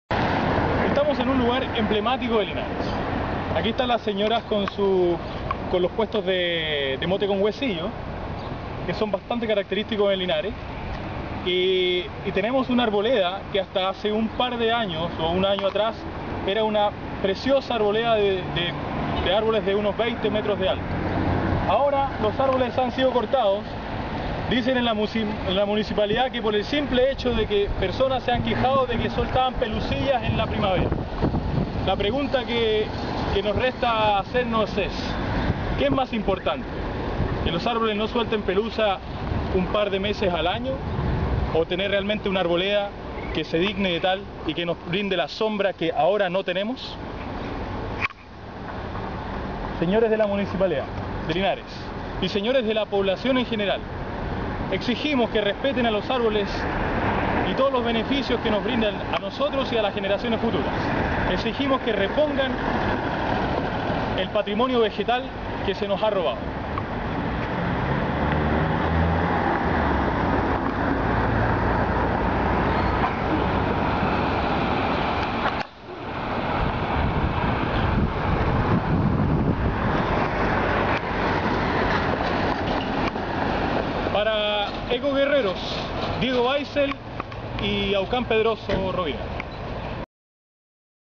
MUTILACIÓN DE ÁRBOLES EN LINARES! ROBO ORGANIZADO DE LEÑA? 01
Este es Linares, Chile. El gobierno local no asesora sus podas con agronomos, paisajistas o ambientalistas. Esta importantísima labor, la poda, en Linares queda a merced de gente inescrupulosa que no se identifica, y con la complicidad o nó de servidores públicos, cortan grandes partes sanas de los árboles que se llevan en sus camiones.

POR FAVOR PARA AYUDAR EN ESTA LABOR DE DENUNCIA Y EDUCACIÓN AMBIENTAL Visita, Comenta y Gusta la página BASTA DE MUTILACIÓN

Cámara: Aucán Pedroso Rovira